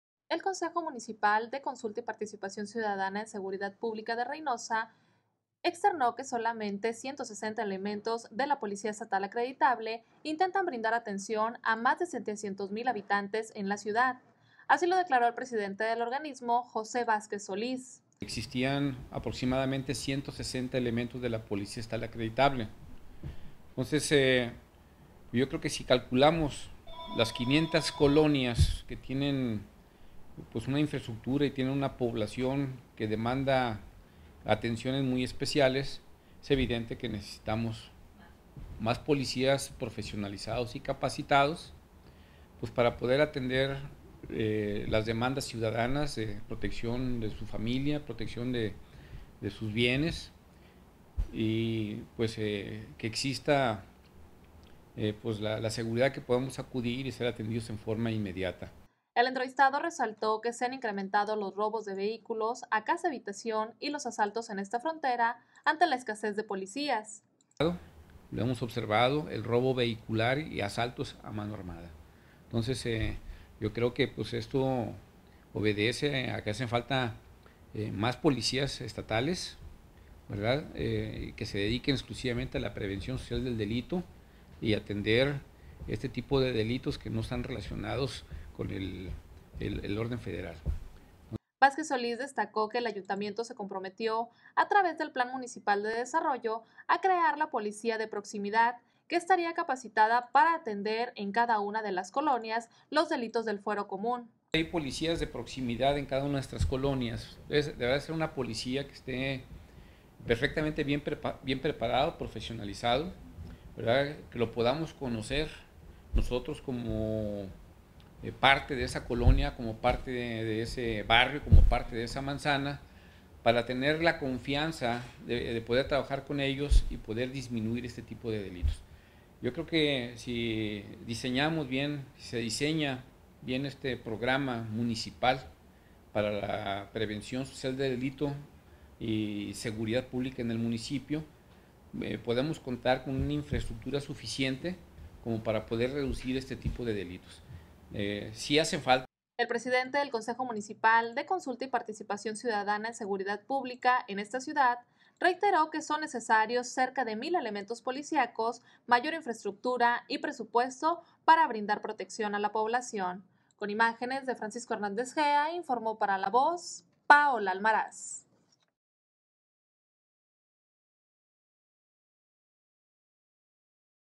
Hacen Falta Policías
Con 160 elementos Policiacos intentan brindar Seguridad a 500 colonias en Reynosa.